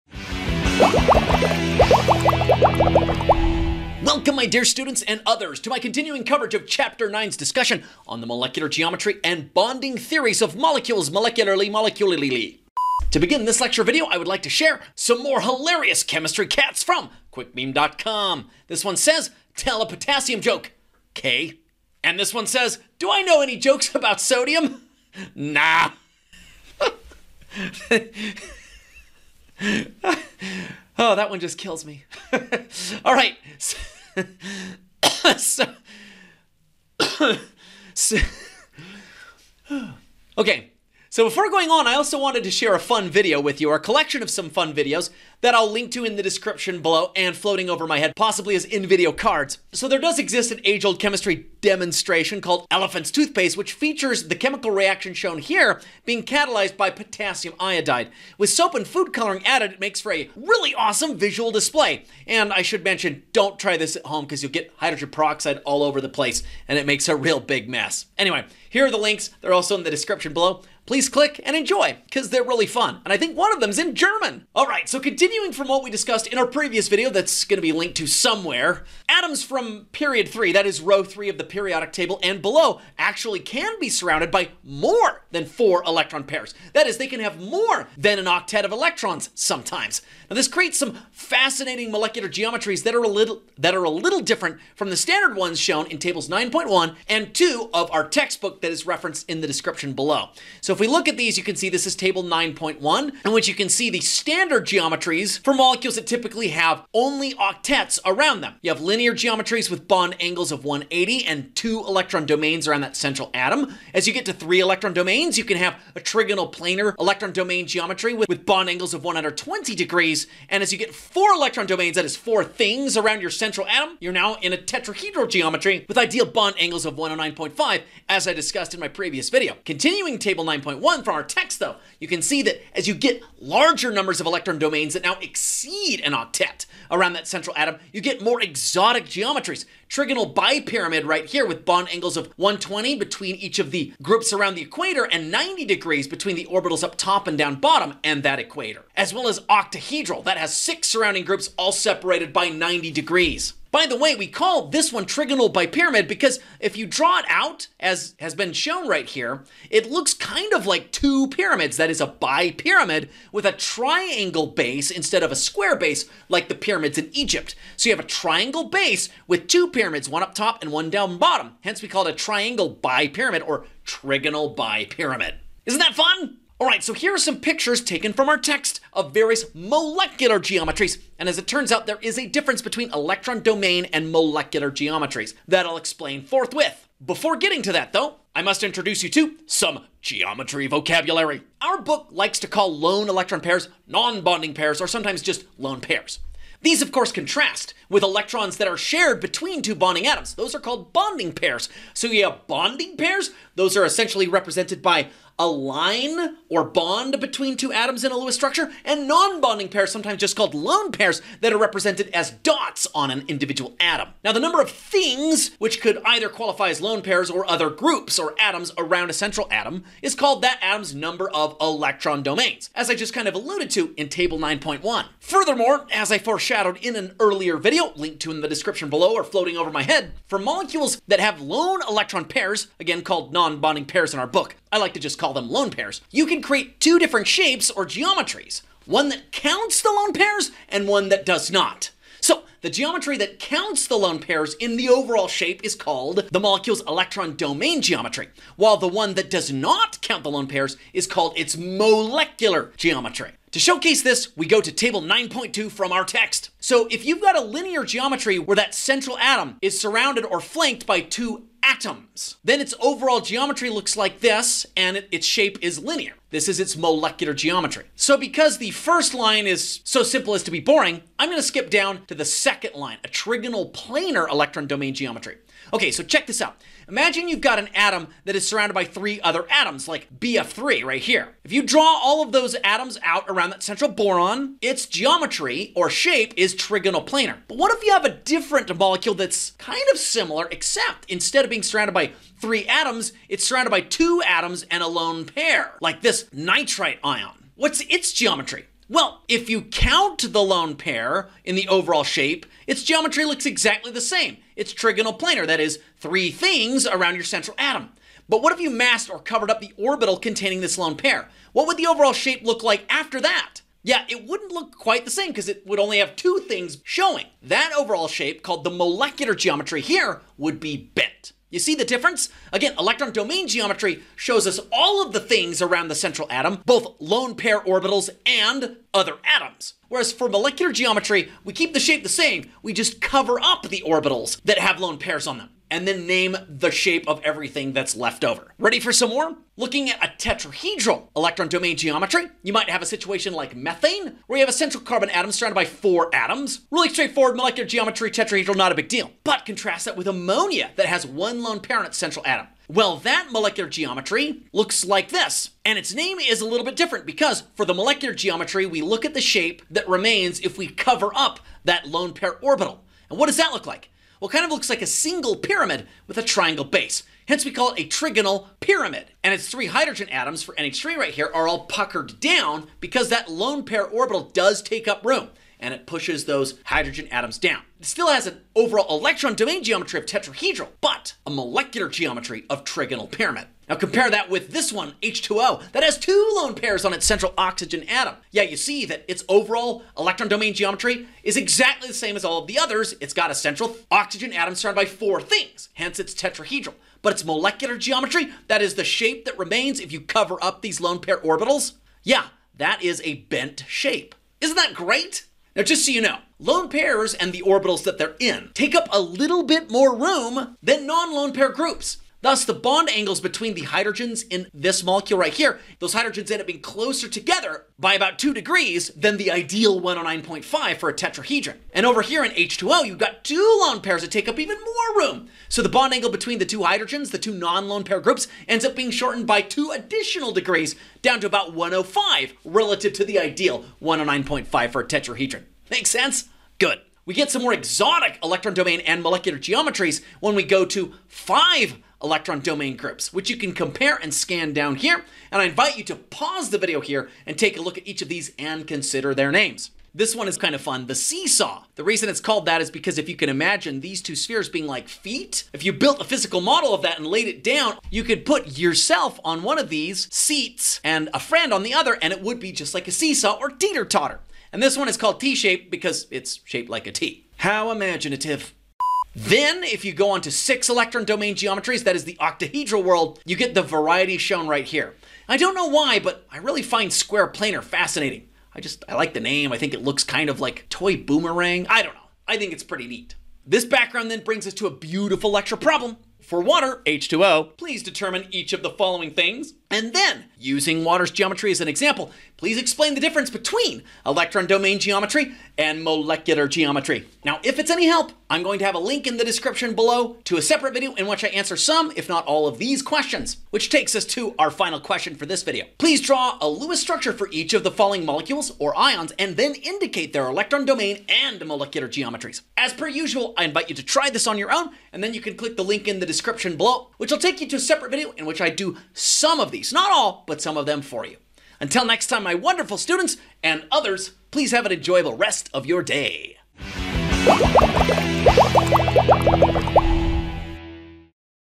Chapter 9 – Part 2: Molecular Geometry vs. Electron Domain Geometry
In this video I’ll teach you more about molecules’ molecular geometries (shapes) and explain the difference between molecular geometry and electron-domain geometry.

Our class textbook: Chemistry: the Central Science, 12th edition, by Brown, Lemay, Bursten, Murphy, and Woodward, ISBN-10: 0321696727 | ISBN-13: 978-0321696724